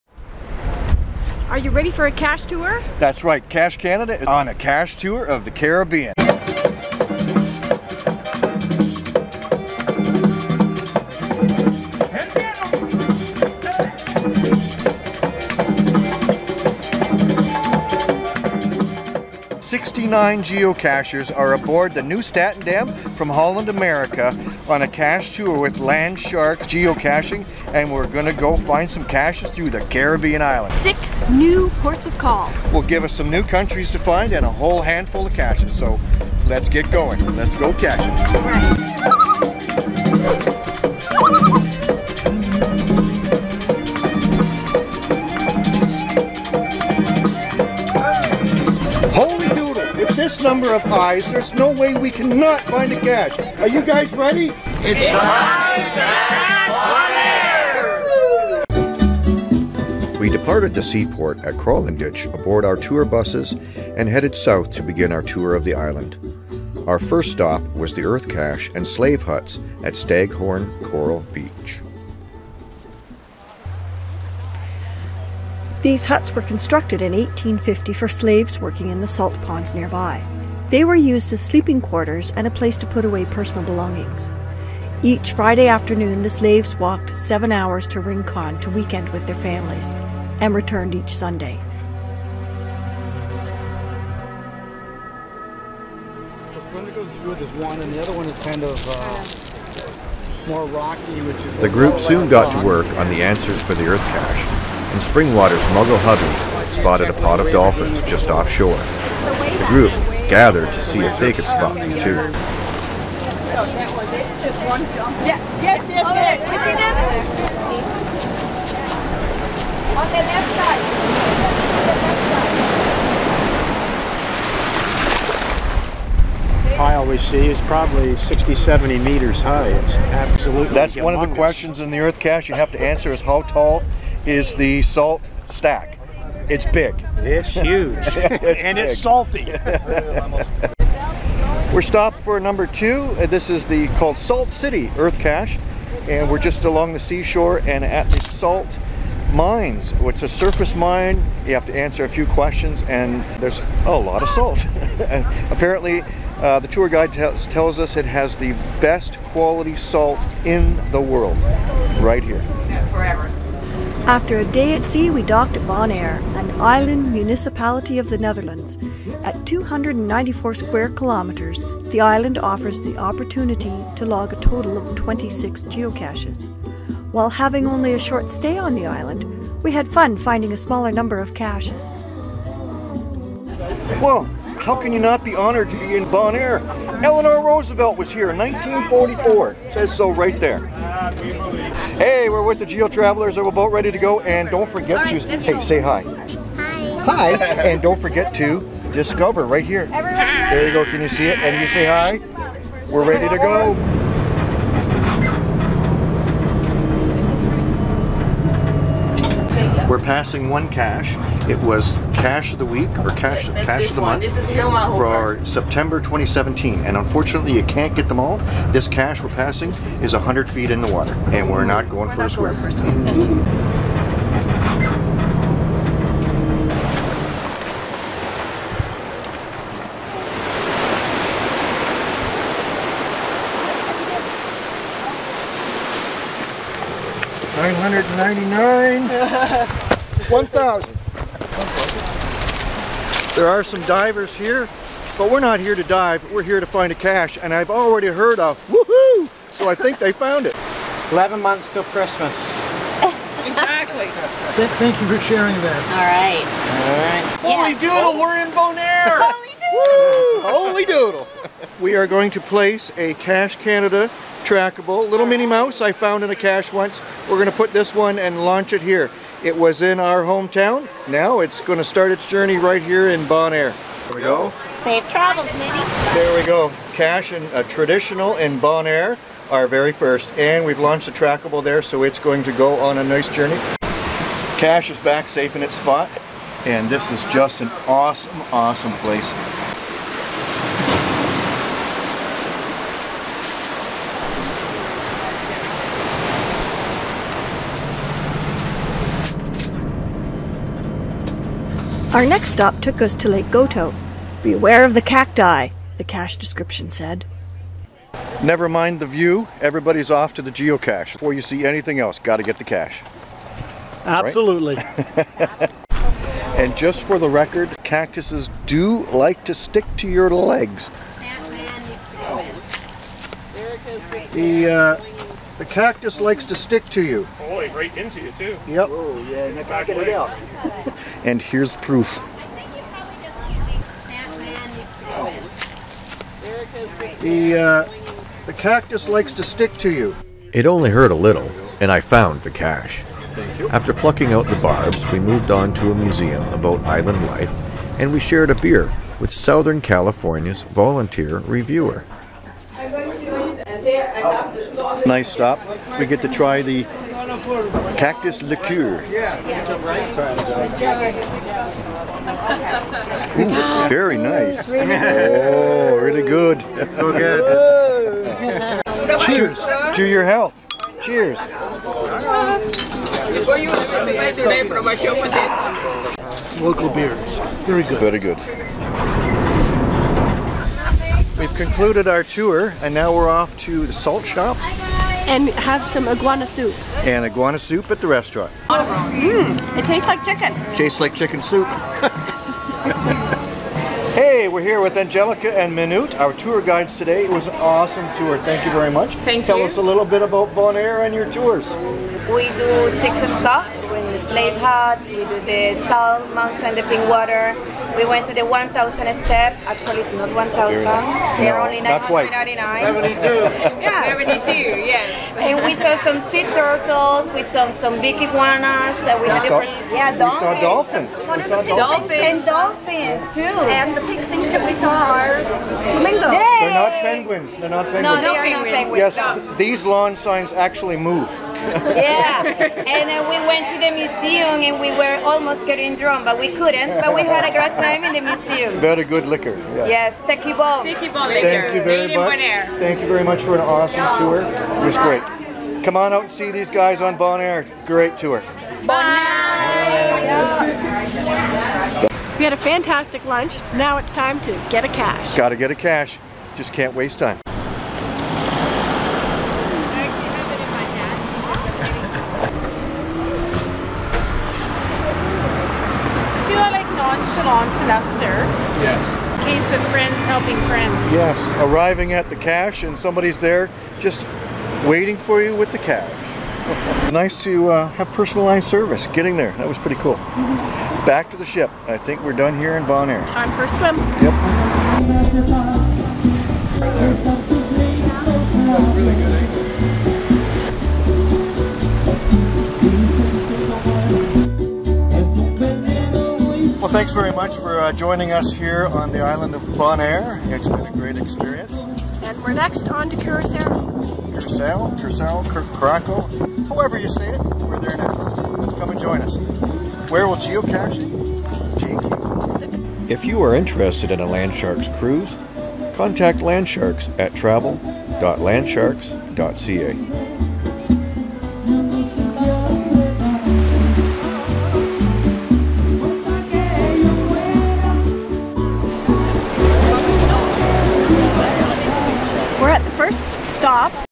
Caribbean Cache Tour - Episode 3 - Bonaire
Cache Canada caches the Caribbean. We're on a Caribbean Cache Tour with Landsharkz and Holland America to cache the islands of the caribbean. This third episode takes us to the island of Bonaire and we get to see a salt mine, walk a thousand steps to a Caribbean Sea cache and londonwesty experiences first hand the barbs of a cactus.